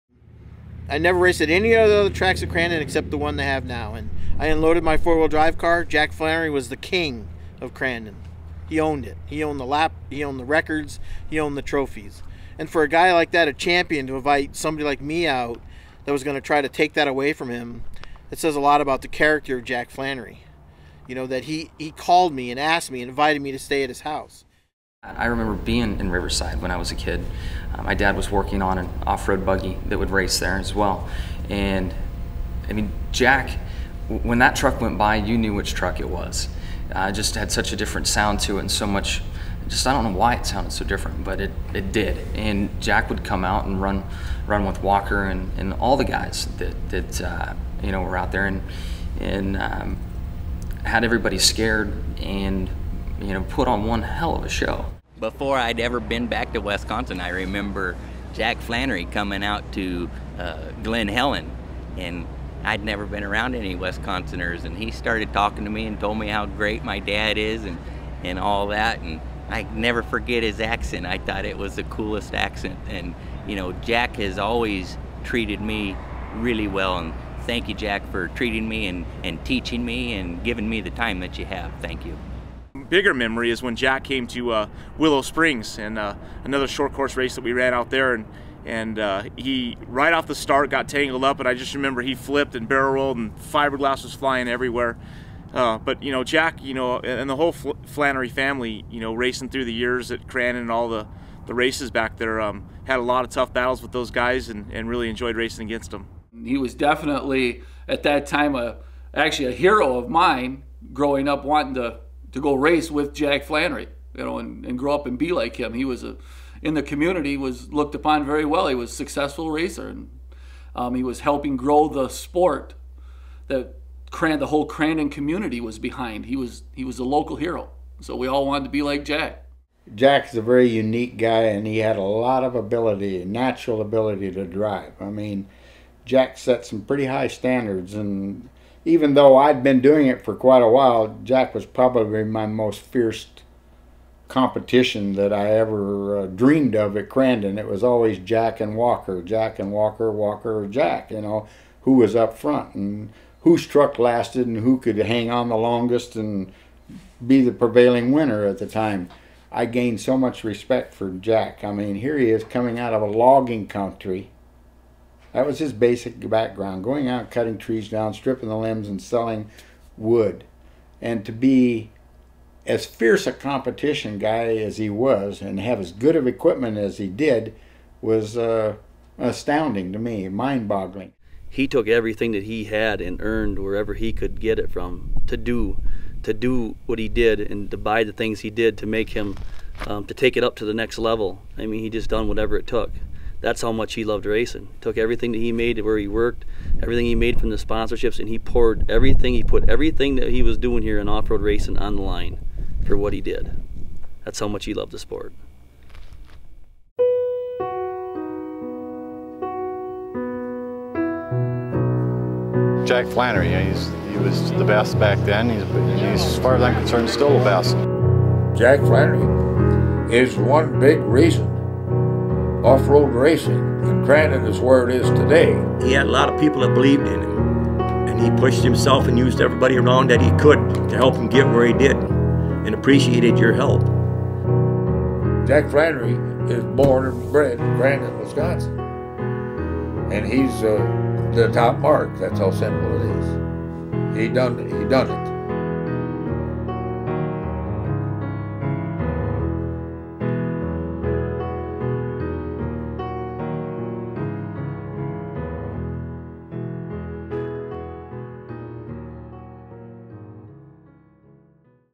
Tribute to Jack Flannery
Jack Flannery was an American off road racer who passed away Tuesday April 6, 2010.  In 2009 Jack was inducted into the Off-Road Hall of Fame.  For more info about Jack Flannery check out the following links: